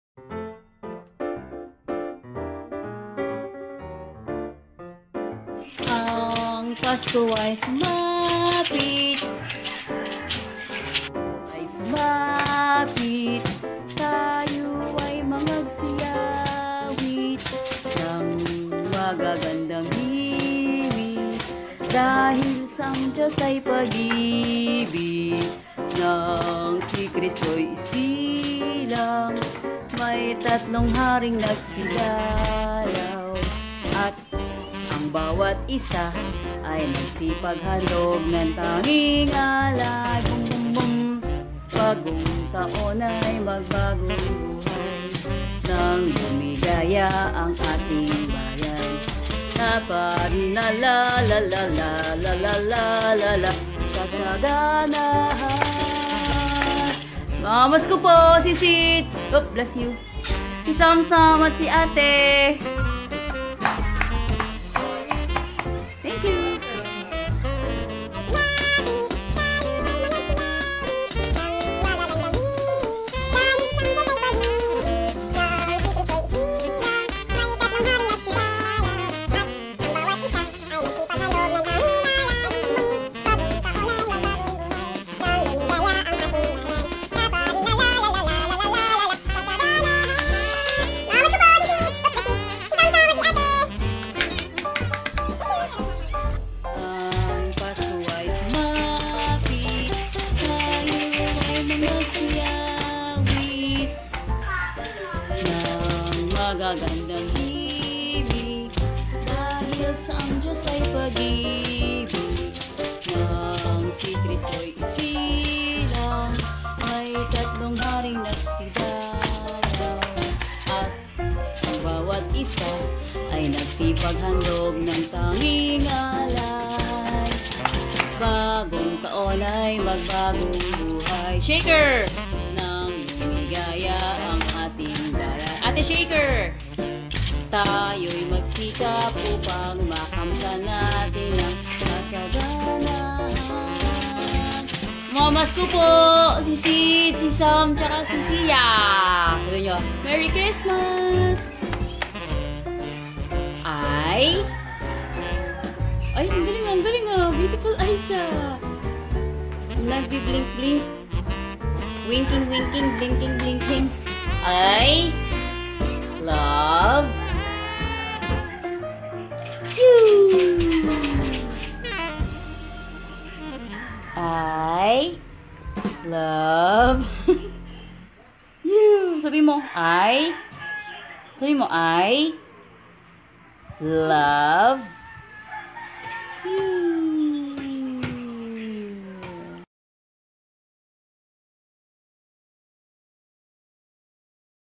Kids' Carolling
Love my kids when they all play their fave instrument! How about you?